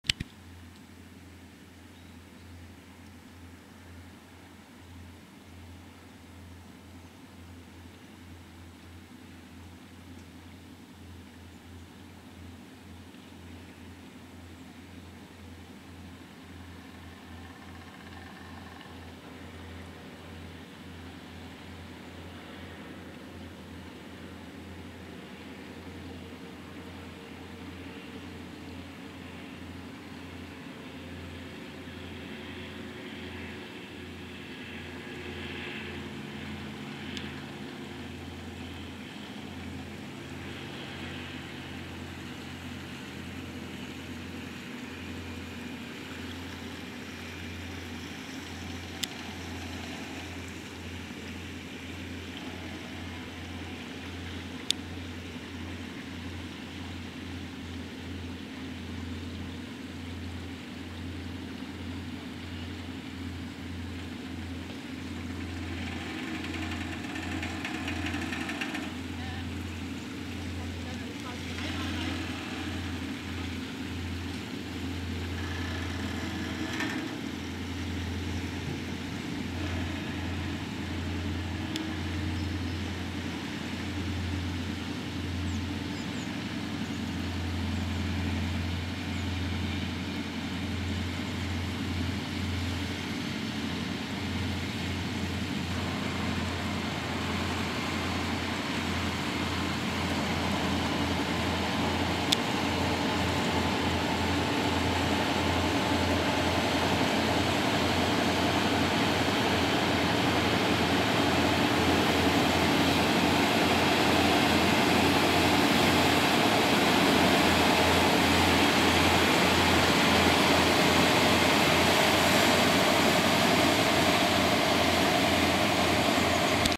Argo schiebt Kohle für Brandenburg
Hier wird das Fluttor Wasbüttel passiert, später ging es dann im Mittellandkanal nach Osten.